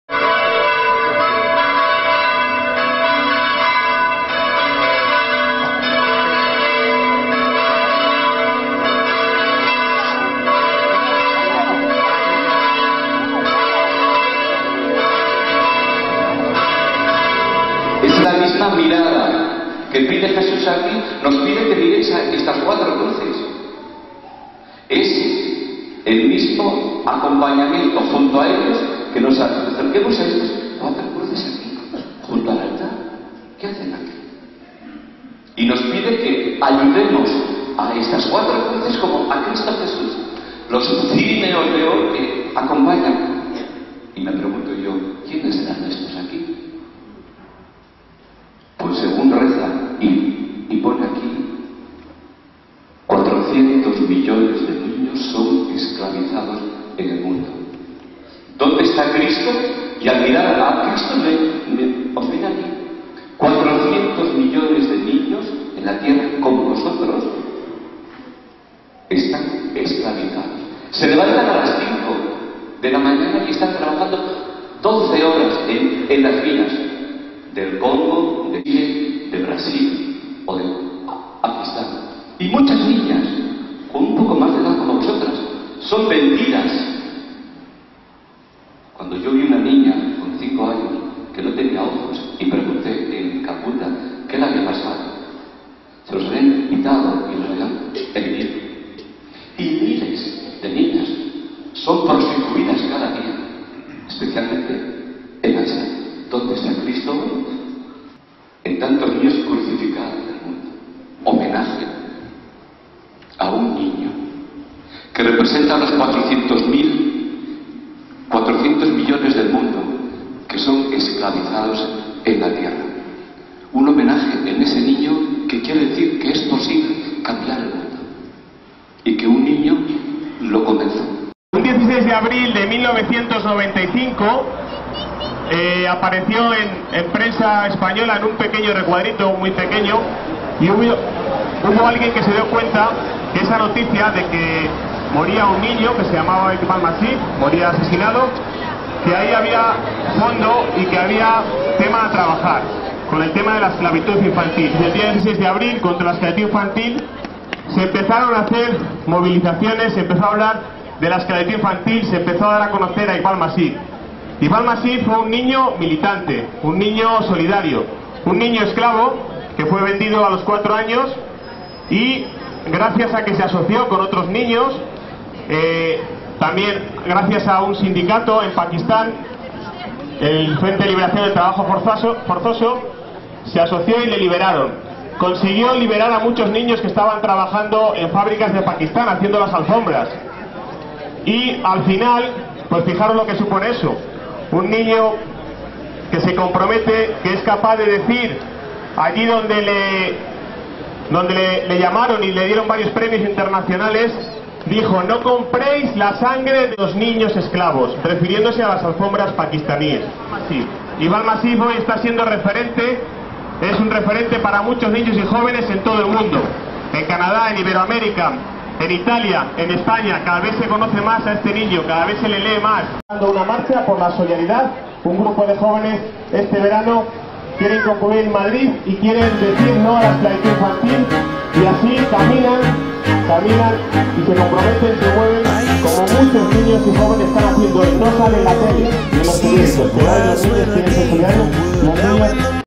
Homenaje a Iqbal Masih Domingo de Ramos
El pasado domingo de Ramos, la parroquia San Francisco Javier de Pamplona, quiso unir la celebración de ese día con la realidad de la esclavitud infantil. Además al terminar la celebración realizaron en la puerta de la Iglesia un homenaje a Iqbal Masih, niño esclavo que fue asesinado a los 12 años por luchar contra la esclavitud infantil.